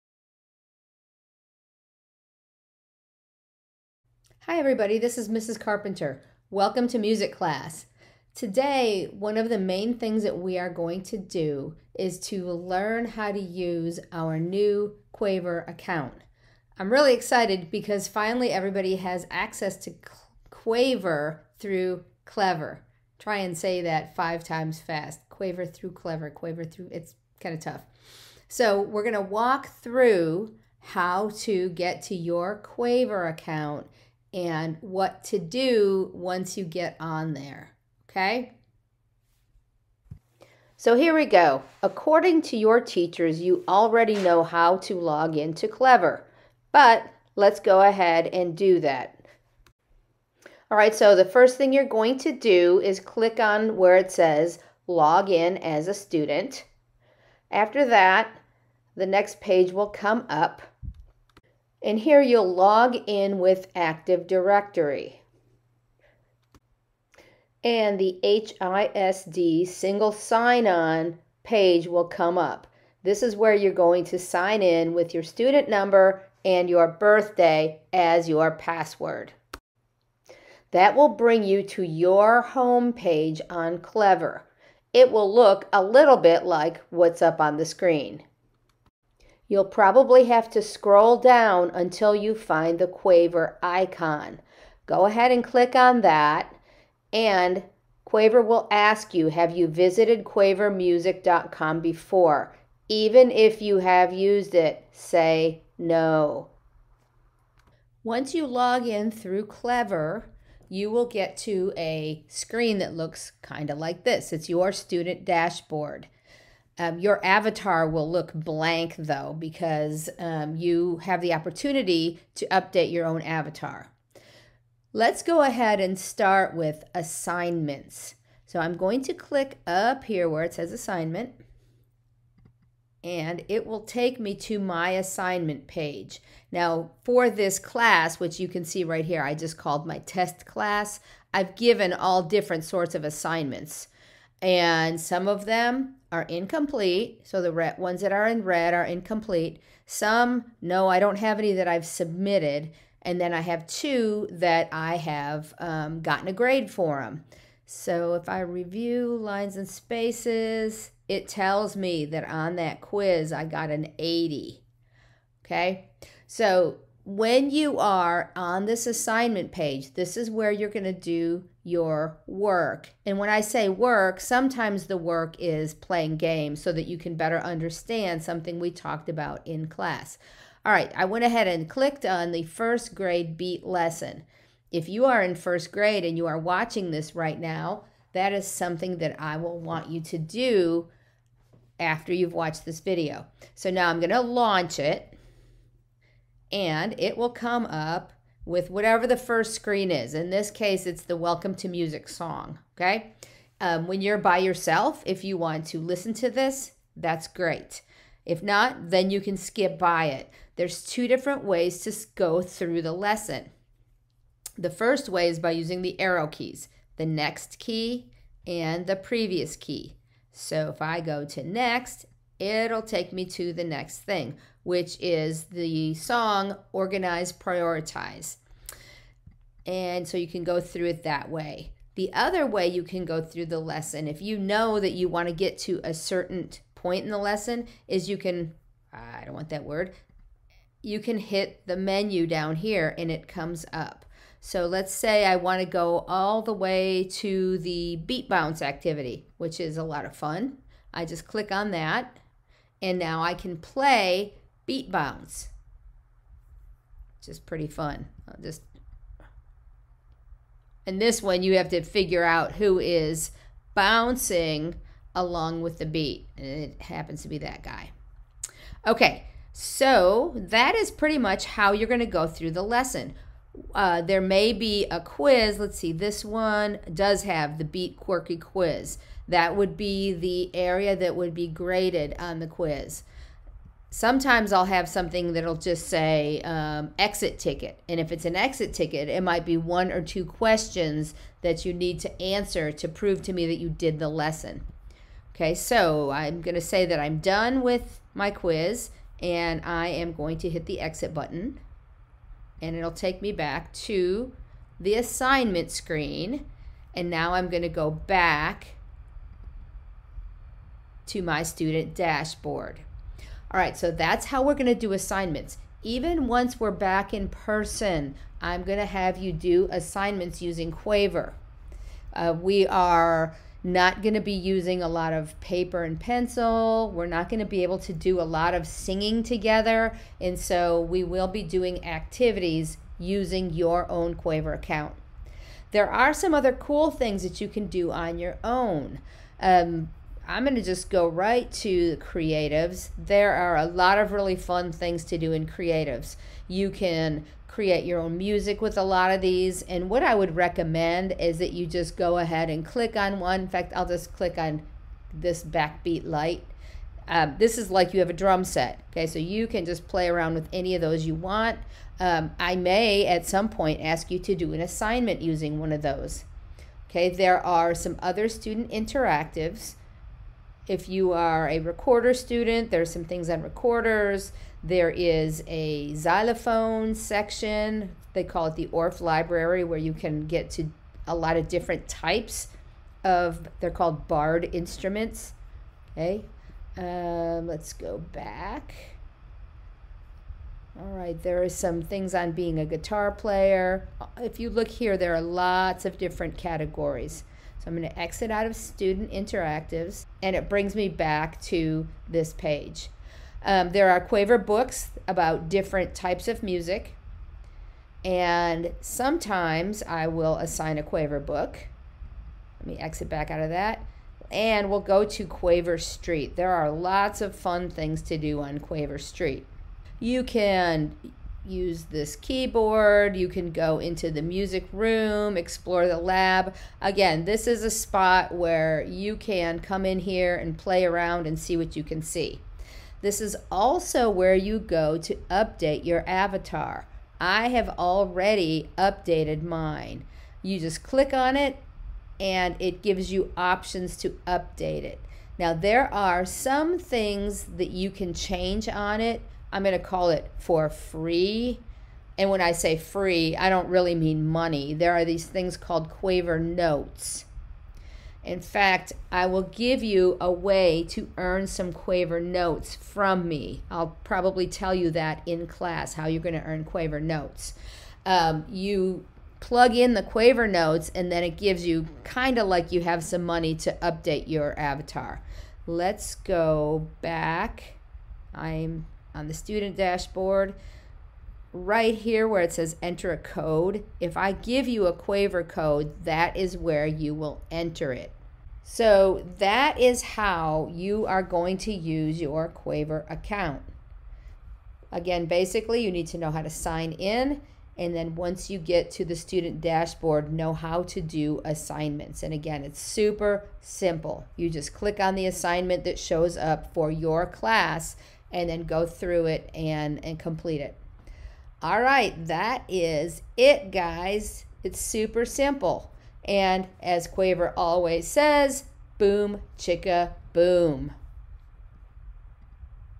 How to use your Quaver Account
This video will show you how to sign on to Quaver using Clever as well as how to use your Quaver Account to do assignments and get creative.